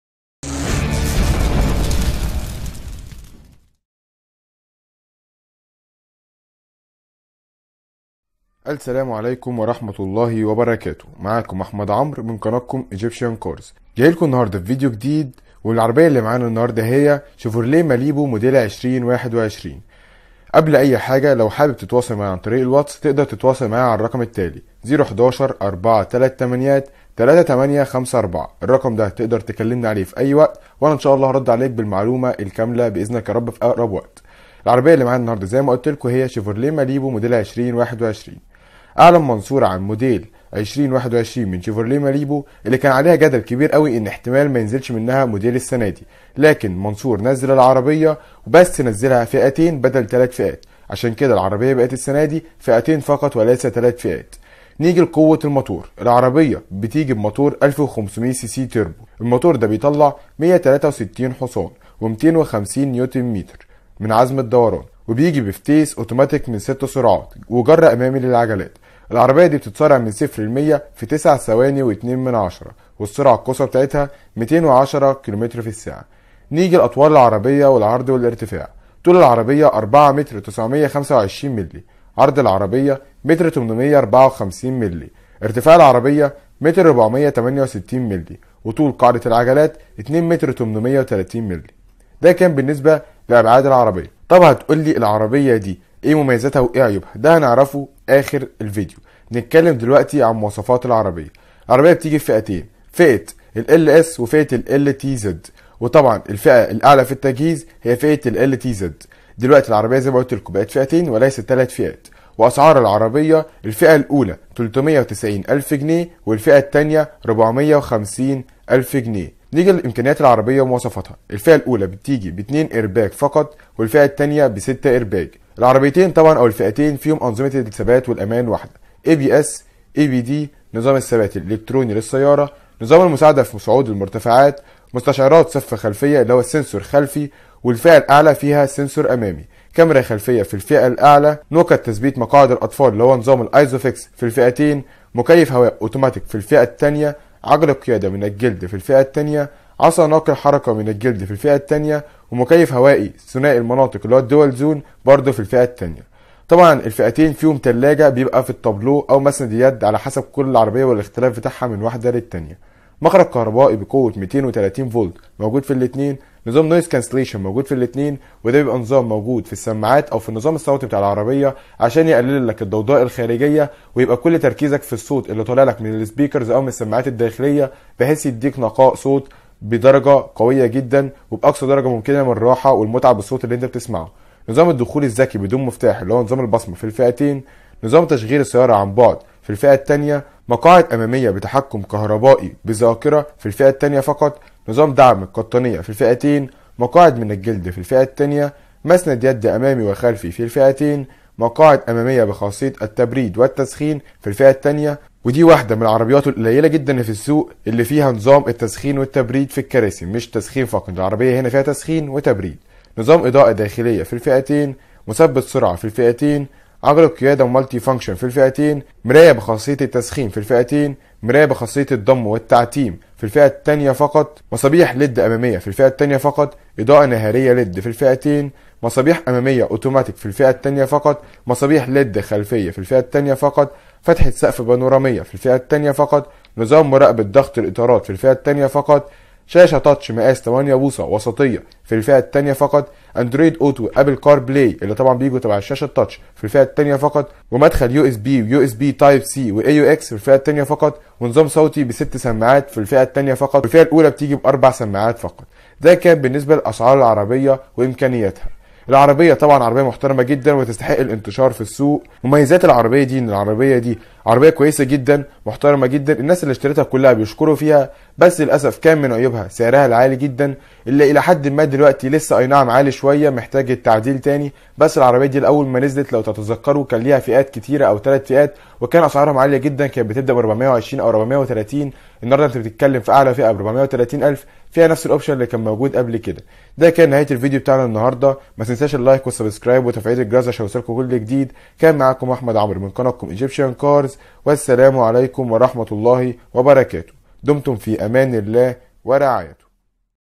عاااجل ظهور ماليبو 2021 بفئات و اسعار جديدة بالكاامل-malibu 2021
سعر العربية بعد التسعير الجديد:
الفئة الاولى:٣٧٠ الف
الفئة التانية:٤٣٠ الف
العربية اللى اول ما نزلت ظلمها سعرها و ظلمها التوكيل للاسف مع انها عربية محترمة جدا و تستاهل المنافسة و بقوة كمان بسبب قوة موتورها و قوة ادائها و قوة امكانياتها و دلوقتى تقدر ترجع تانى للمنافسة باسعار و فئات جديدة كل ده هنعرفه من الفيديو
و زى ما وعدت حضراتكم اى استفسار تقدروا تكتبوه فى الكومنتات او من خلال الاستفسار من خلال الواتس على رقم:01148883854
و هرد على حضراتكم فى اقرب وقت بالمعلومة كاملة و هتبقى اكيدة ان شاء الله
دمتم فى امان الله و رعايته
بشمهندس/احمد عمرو